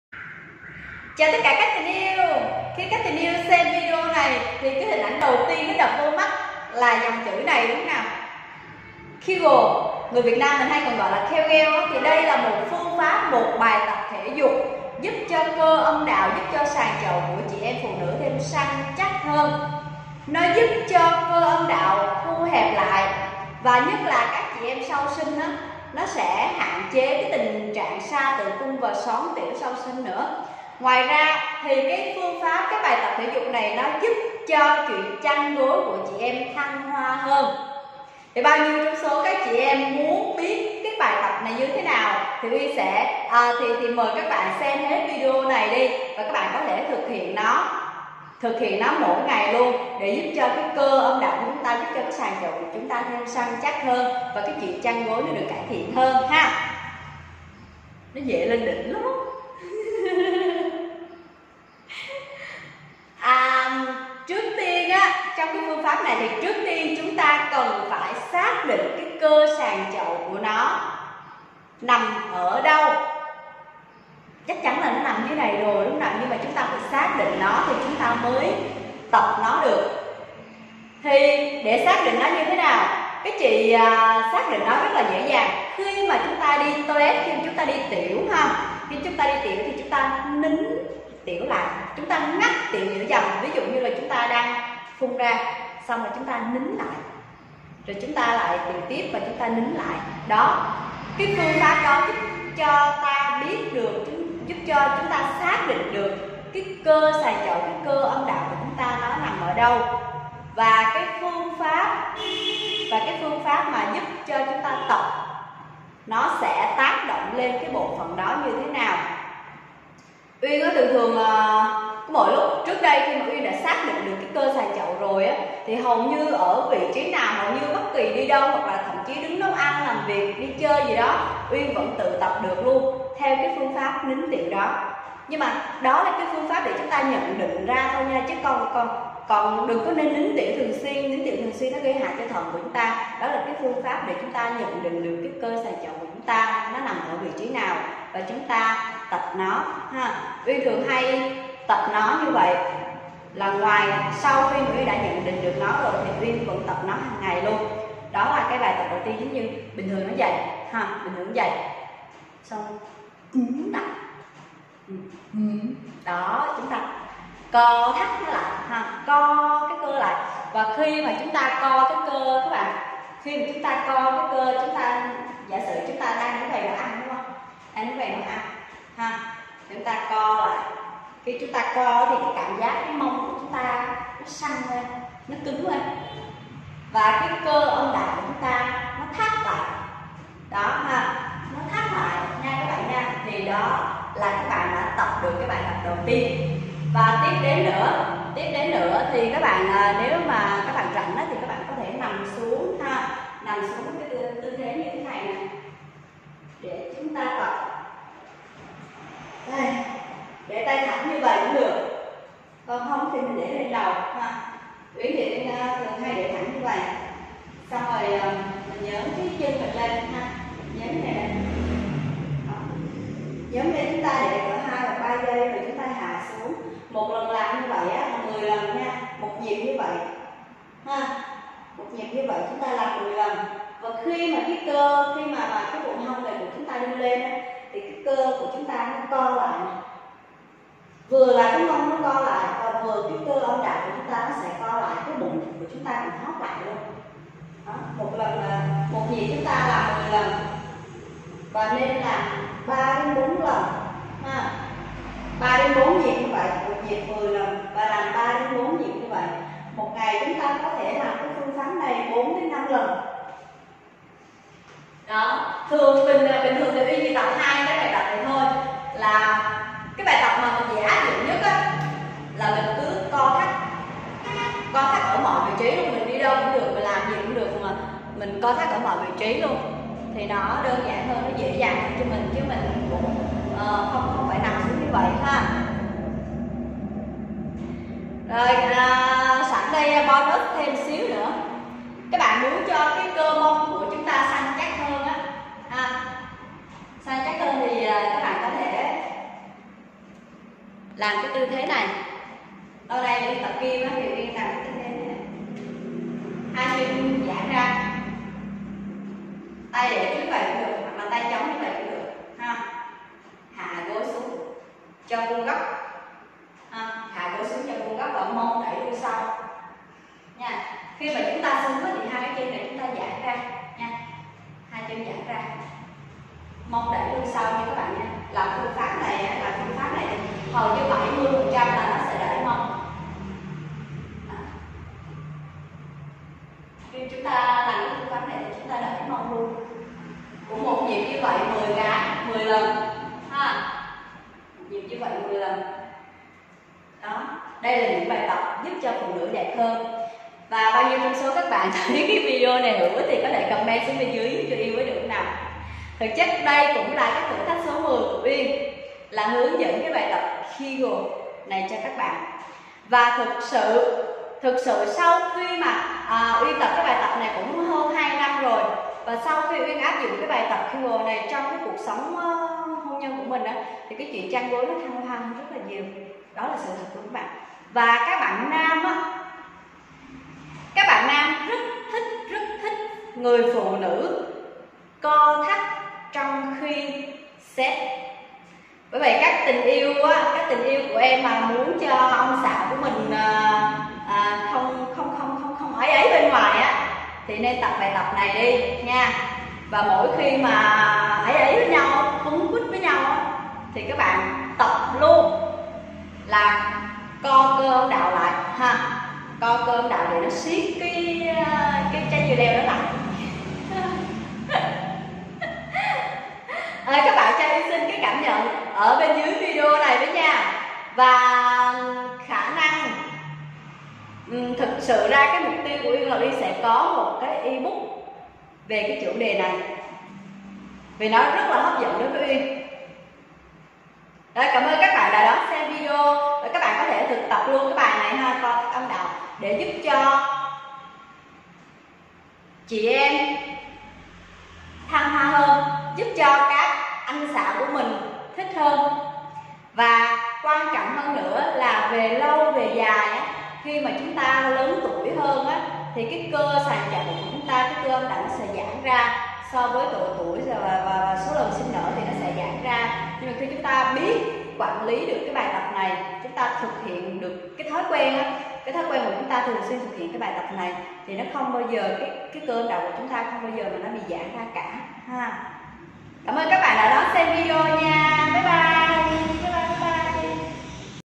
PHƯƠNG PHÁP NÀY SẼ GIÚP CHUYỆN CHĂN GỐI CỦA BẠN TUYỆT VỜI HƠN.
Kegel: là bài tập thể dục dành cho nhóm cơ mu cụt, nó giúp cải thiện sức khỏe tình dục cả nam và nữ.
Ngoài ra nó là bài thể dục cực kỳ tốt cho các mẹ sau sinh.

Bao nhiêu trong số các bạn đã từng nghe đến bài tập này rồi nhỉ?

Hãy xem video bên dưới nhé😉